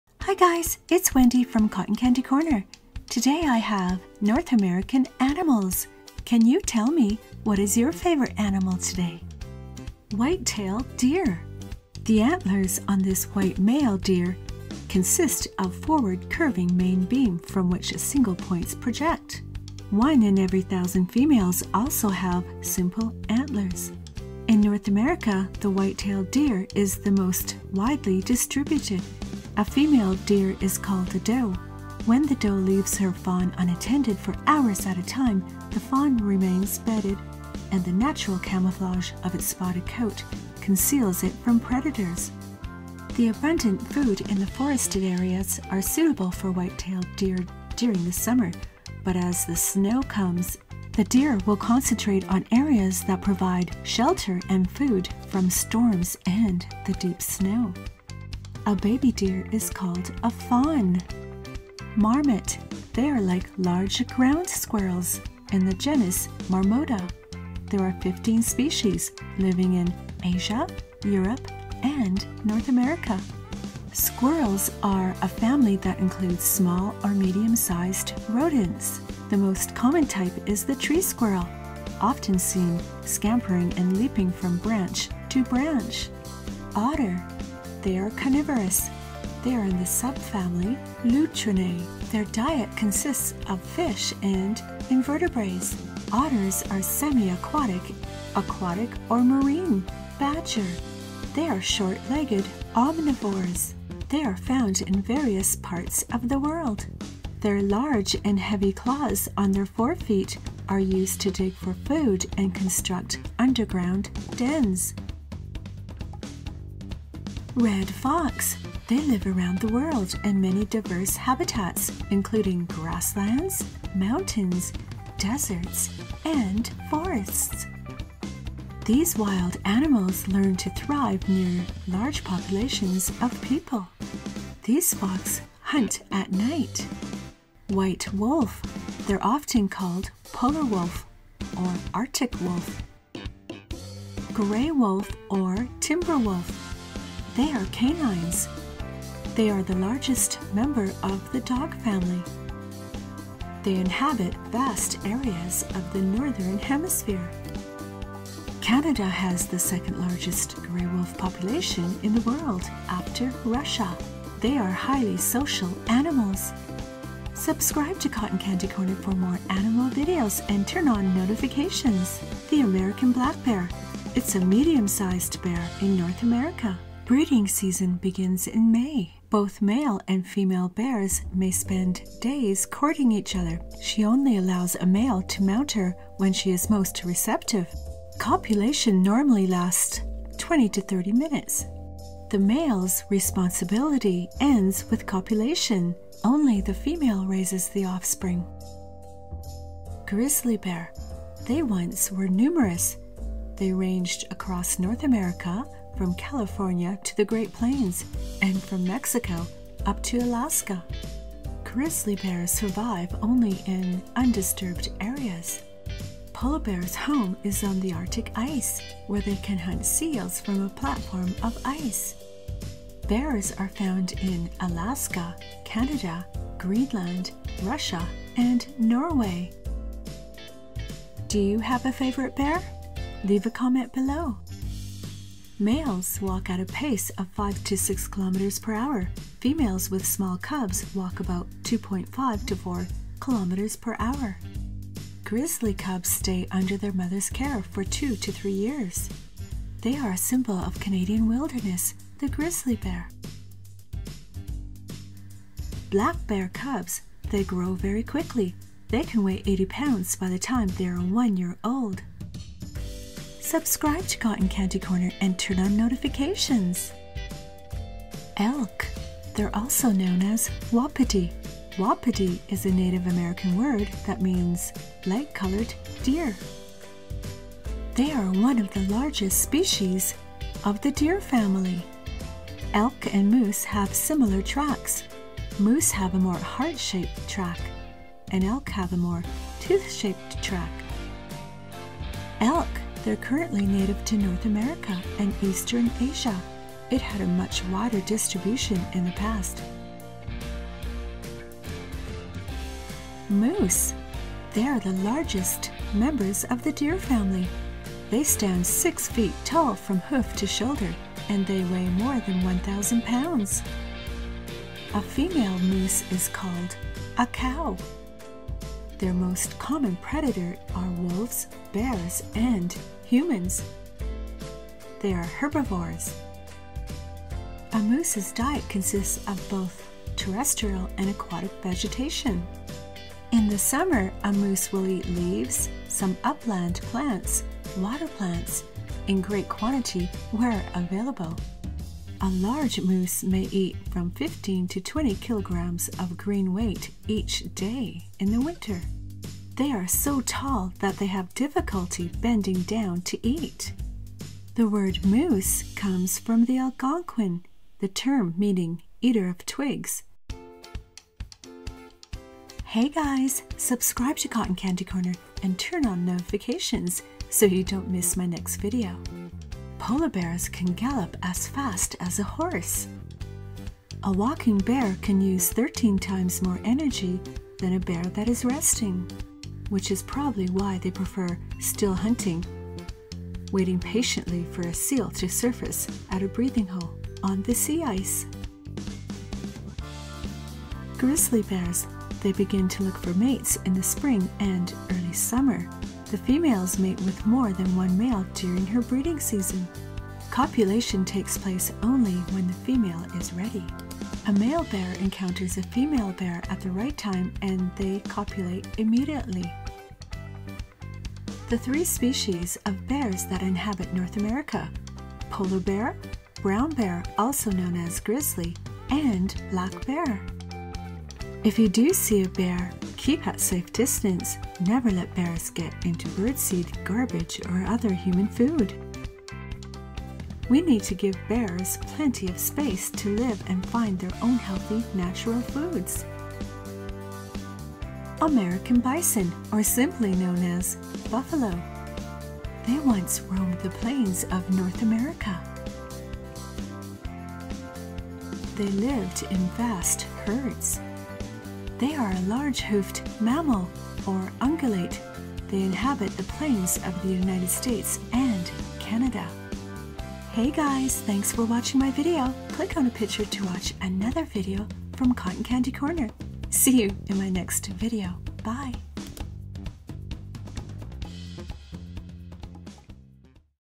North American Animals - Polar Bear, Grizzly Bear, Black Bear, Moose, Deer 13+
North American Animals - Polar Bear, Grizzly Bear, Black Bear, Moose, Deer 13+ My favourite animal in today's video are the bear and moose.

A marine mammal, the polar bear spends many months of the year at sea.

The American black bear is the most commonly seen bear in North America

Grizzly bears eat meat and fish, their main diet consists of berries and plants.

North American Brown bears include:
Dall Island brown bear
Alaska Peninsula brown bear
Kodiak bear
Sitka Brown bear
Stikeen Brown bear
Ungave Brown bear (extinct)
Californian grizzly bear (extinct)
Mexican Grizzly bear (extinct)